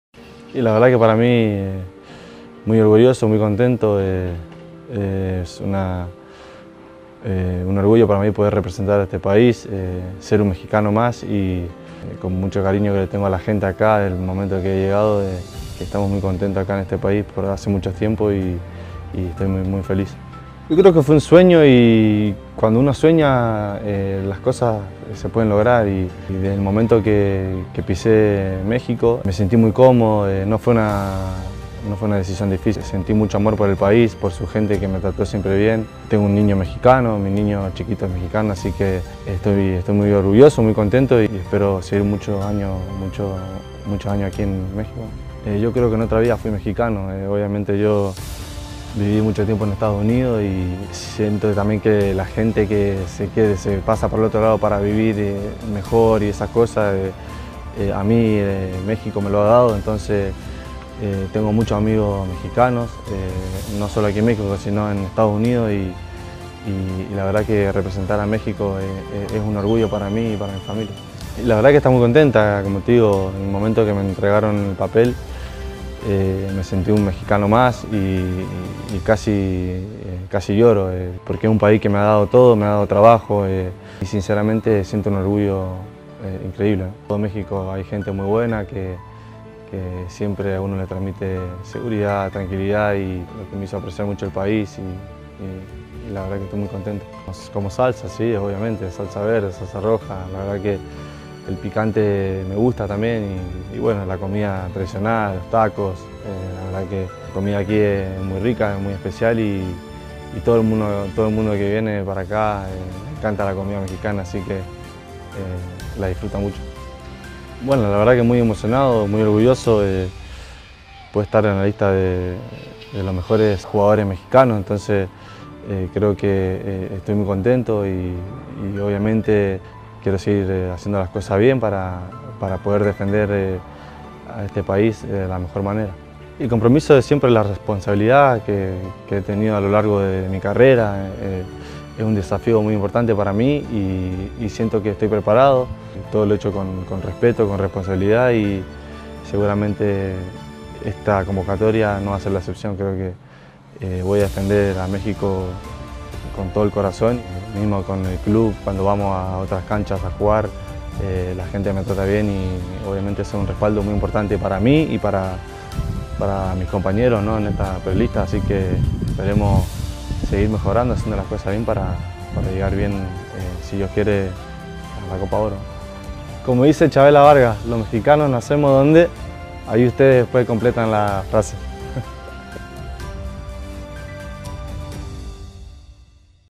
"Es un orgullo para mí poder representar a este país" - Rogelio Funes Mori
Rogelio Funes Mori reportó por primera vez con la Selección Nacional de México 🇲🇽  y dio estas bellas palabras con Rayados👏

Créditos: @Rayados